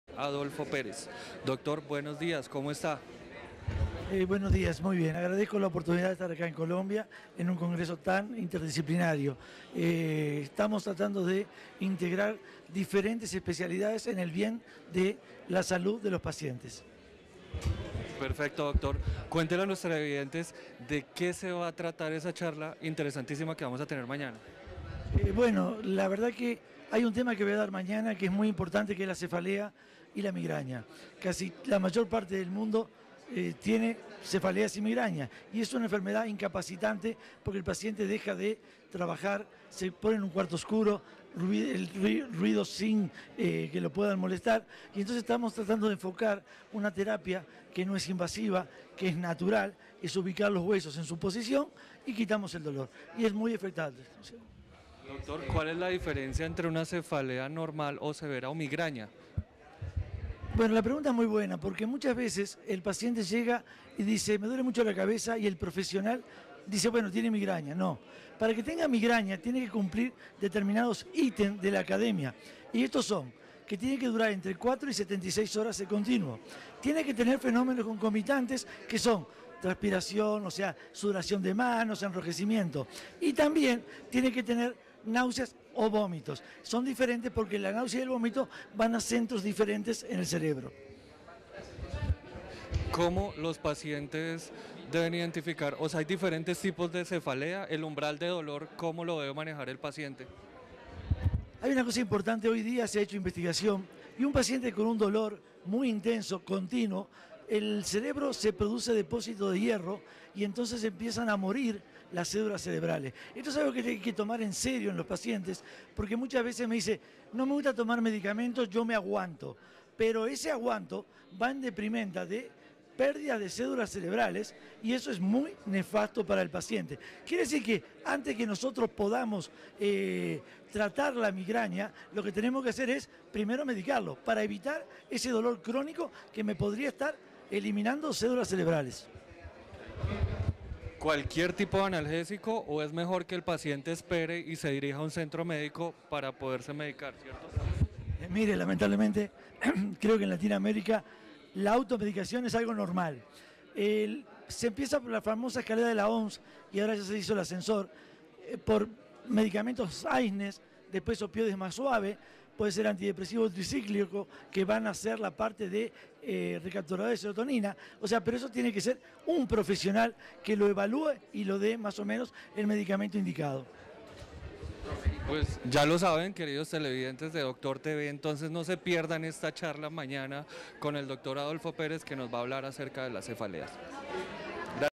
Entrevista SIISDET Dr. Adolfo Pérez Brignani, ODT. Estomatología. Dolor Facial.
II Congreso Internacional Médico Científico y Desarrollo Empresarial en Salud - SIISDET "Integramos la Salud Mundial" realizado dentro de las instalaciones de la Universidad Militar Nueva Granada en la ciudad de Bogotá. D.C. Colombia. Mayo 10 - 12 del 2018. Educación Médica Continuada Multidisciplinaria con aportes internacionales de Conferencistas Invitados iconos en el mundo.